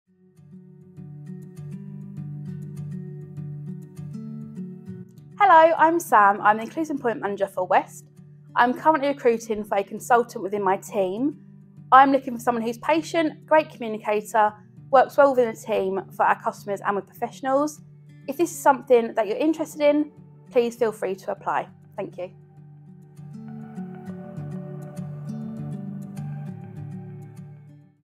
Meet The Manager | Inclusive Employment Consultant
Sam Goldsmith, Inclusive Employment Team Manager gives us an introduction to the role - Inclusive Employment Consultant.